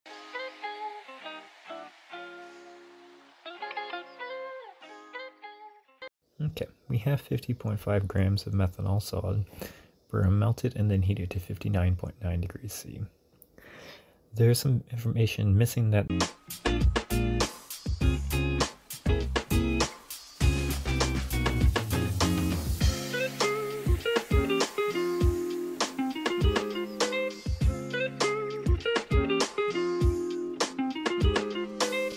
Calculate the amount of heat needed to melt 50.5 g of solid methanol (CH3OH) and bring it to a temp…
Calculate the amount of heat needed to melt 50.5 g of solid methanol (CH3OH) and bring it to a temperature of 59.9 Â°C. Be sure your answer has the correct symbol and the correct number of significant digits.